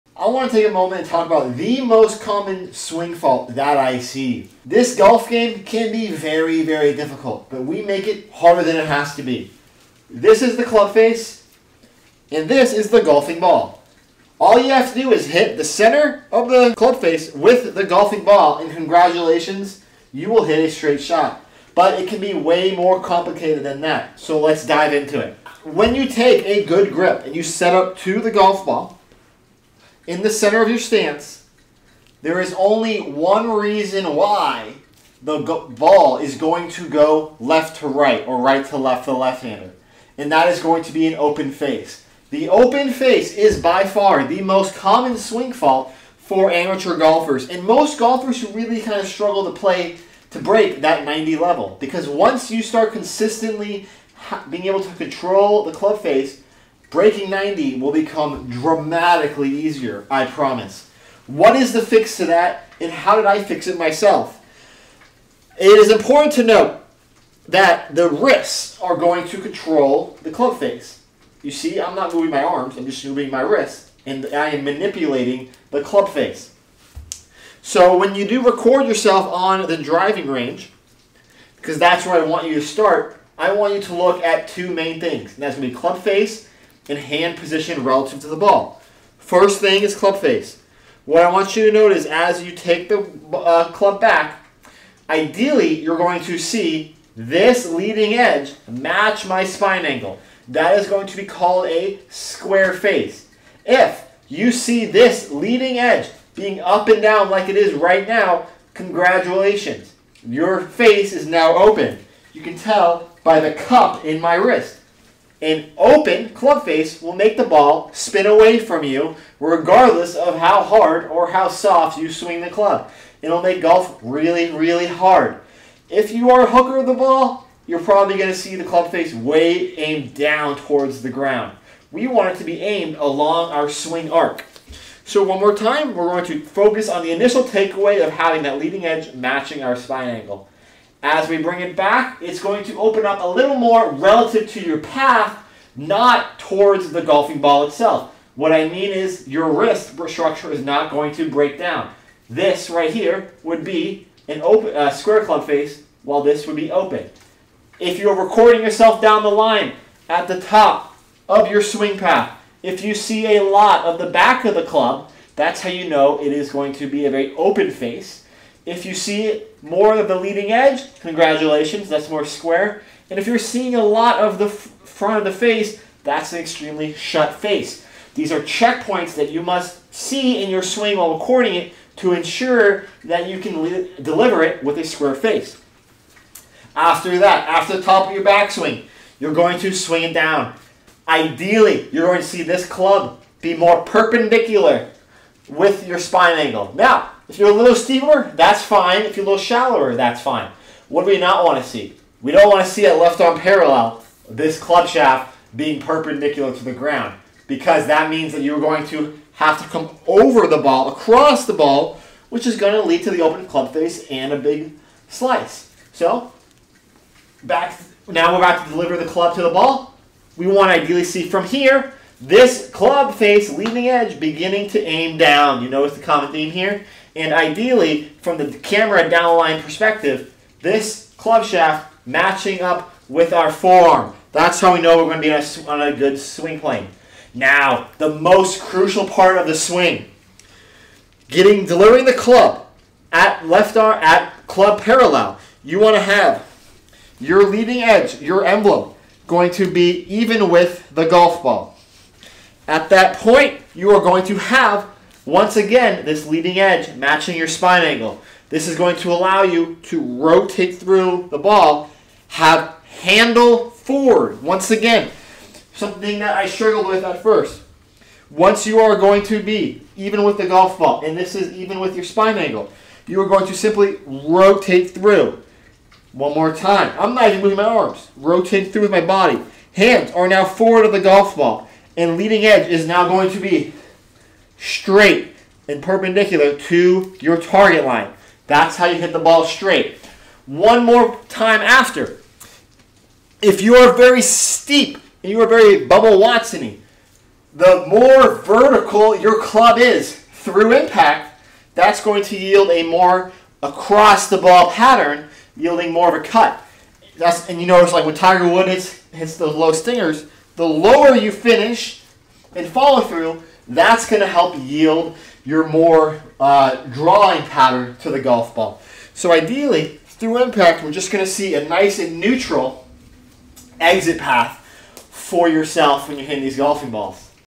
This is the most common swing fault in golf! | MUST fix
Online Golf Lessons!!

How to stop slicing the golf ball. How to square the club face. How to keep your hands forward at impact.

Golf Gear!

Range Finder:

Golf Glove:

Indoor Putting Practice: